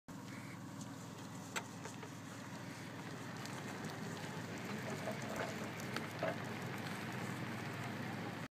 Kaitlyn as ladybug & Grayson as Mickey Mouse
Trick-or-Treating on our town's postponed Hallowe'en